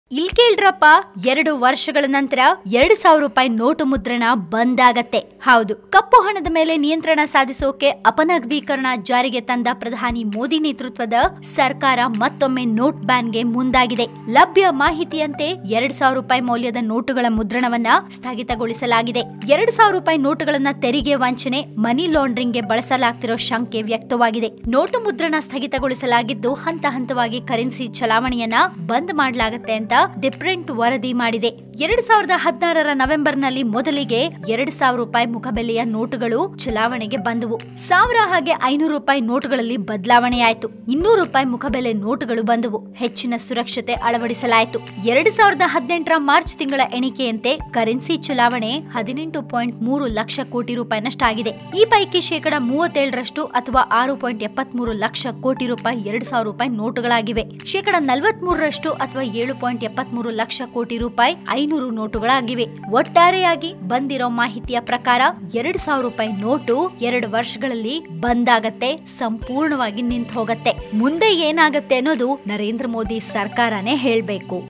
2 ವರ್ಷಗಳ ನಂತರ 2000ರೂ ನೋಟು ಮುದ್ರಣ ಸ್ಥಗಿತ | Oneindia Kannada
India has stopped printing Rs 2,000 notes in a bid to slowly reduce their circulation, a highly placed government source told The Print The cut in circulation does not mean the Rs 2,000 notes will become invalid.

ಕಪ್ಪುಹಣದ ಮೇಲೆ ನಿಯಂತ್ರಣ ಸಾಧಿಸಲು ಅಪನಗದೀಕರಣ ಜಾರಿಗೆ ಬಂದ ಪ್ರಧಾನಿ ಮೋದಿ ನೇತೃತ್ವದ ಸರ್ಕಾರ ಮತ್ತೊಮ್ಮೆ ನೋಟ್ ಬ್ಯಾನ್ ಗೆ ಮುಂದಾಗಿದೆ. ಲಭ್ಯ ಮಾಹಿತಿಯಂತೆ 2000 ರುಪಾಯಿ ಮೌಲ್ಯದ ನೋಟುಗಳ ಮುದ್ರಣವನ್ನು ಸ್ಥಗಿತಗೊಳಿಸಲಾಗಿದೆ.